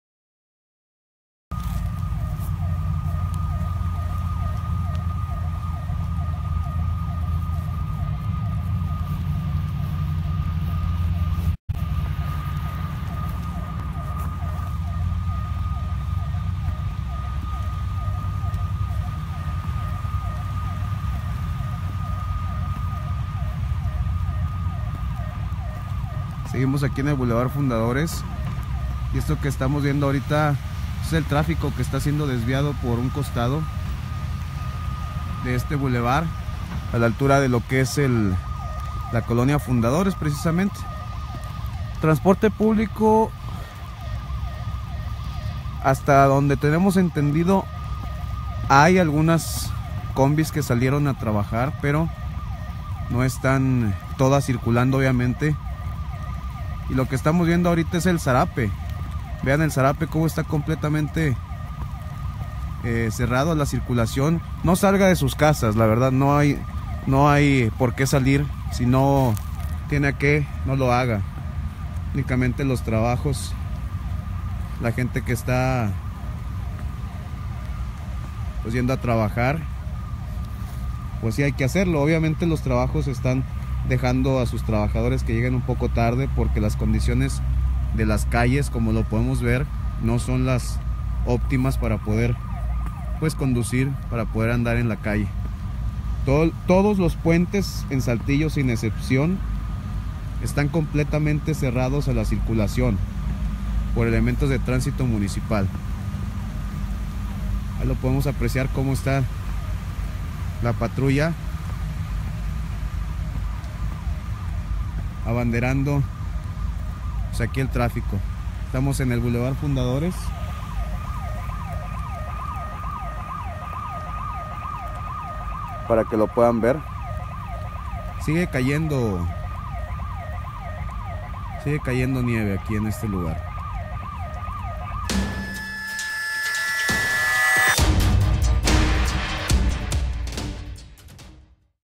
Amanece Saltillo cubierto de hielo y nieve
Fecha: 08/12/2017
Hora: 15:28
Lugar: Saltillo
Tema: Nevadas
Fue en las colonias de Saltillo donde la cubierta de nieve y hielo, alcanzó alrededor de los 5 centímetros.
El día de ayer a las 8 de la noche empezó a lloviznar y caer granizo en la ciudad, así como empezó la presencia de hielo y agua nieve en las colonias más alejadas.
Videos de: Código Rojo Saltillo